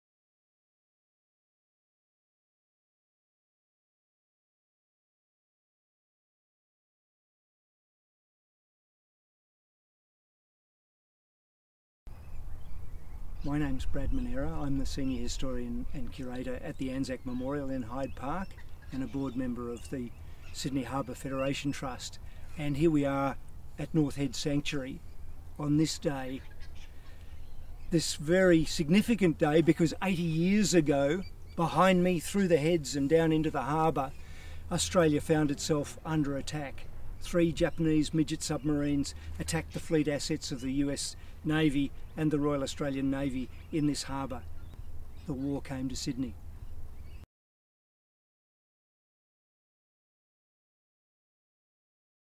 80th Anniversary of the midget submarine attack on Sydney Harbour (31 May - 1 June 1942)
Eighty years ago tonight, Sydney Harbour came under attack from three midget submarines dispatched by the Imperial Japanese Navy. Today, the Harbour Trust acknowledges and commemorates those who were wounded or lost their lives during the attack (31 May to 1 June 1942), including 21 sailors aboard the HMAS Kuttabul. To mark the occasion, we filmed a short video featuring Member of the Trust, Brad Manera, at North Head Sanctuary where Australian servicepeople bravely defended Sydney Harbour during World War II.

Want to visit North Head Sanctuary in person? Plan your trip